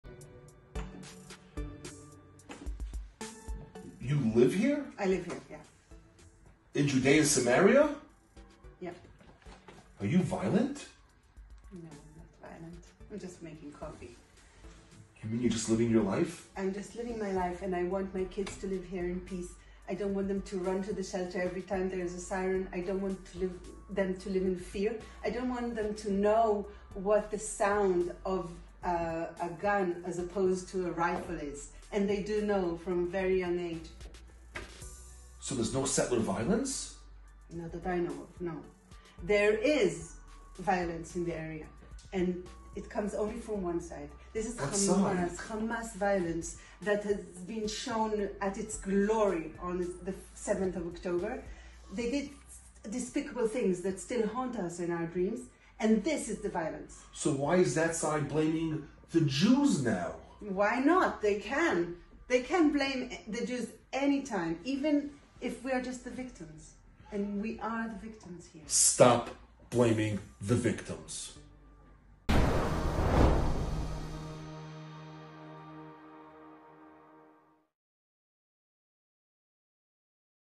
Debunking the myths targeting Judea and Samaria
AFJS board member Gabriel Boxer – the popular blogger known as "Kosher Guru" speaks to the Gush Etzion Foundation's Karnei Eldad, who says the residents of Judea and Samaria just want to live in peace for the sake of their children and their future.
and help spread the truth about the Jewish communities in Judea and Samaria.